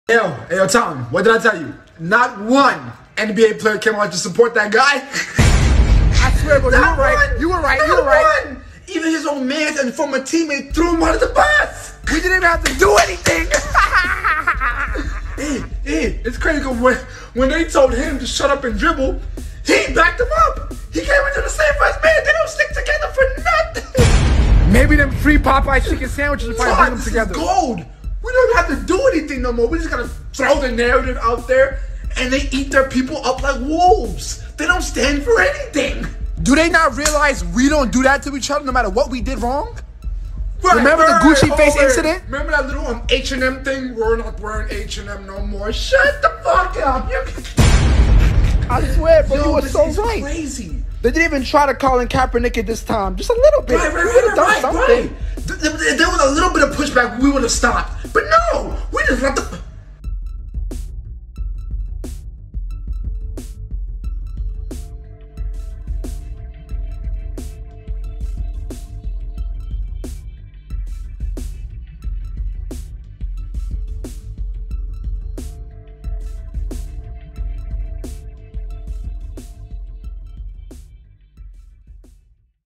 what does Amazon have to do for antisemitic products?!?
The way this scene is soooooo boring and embarrassing at the same time.

shannonsharpe flipped into the punishments for KyrieIrving being too steep & after JaylenBrown issuing support having been pressed about a subsequent tweet in need of a clarification tweets to go on, the entire NBA is full of cowards

athletic types are the gladiator of today with the actual power to cut their ties from the elite affluent in a matter of moments but don't

Just think how easily the NBA alone could be completely rendered ineffective if just 50% of the BlackMen walked. superstar or otherwise an established professional can NEVER have his talent discredited, especially since some non draft players have phenomenal careers even over 1st round picks

Then the WillieLynch itbeyourownpeople of shaquilleoneal charlesbarkley & lebronjames which the latter was defended by Bro. Irving when told to shutupanddribble going on record against him, when nocomment was the move is a wegottadobetter moment

BlackLeadership has failed the BlackCommunity just as the church has however comma the BlackPeople cancelculture participation is wild when it's those being done wrongfully

Go grab some merch! We sell merchandise so we don't have to sell our soul.

We do find it hilariously amerikkka to associate the hebrewisraelites to Ky but not jan6 to donaldtrump

PS the NFL hasn't shared any data on jerryjones no deep dive or anything like that